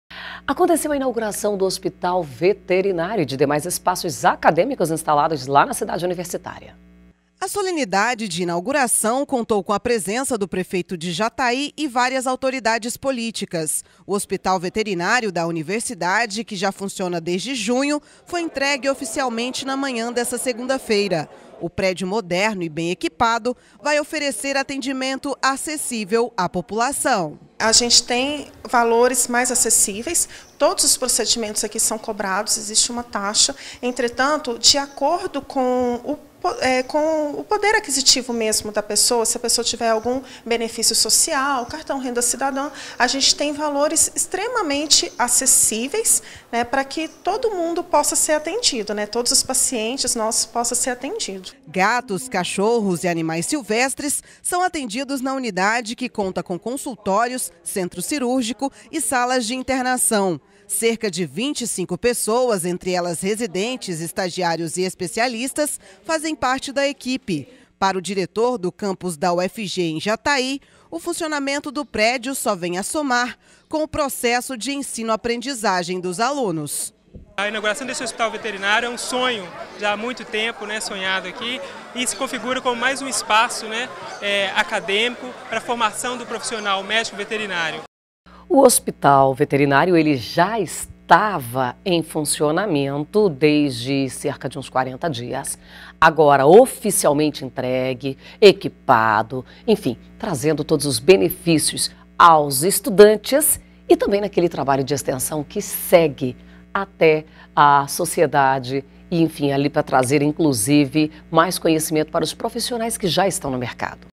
Hospital Veterinário
Inaugurado hospital veterinário da UFG em Jatai.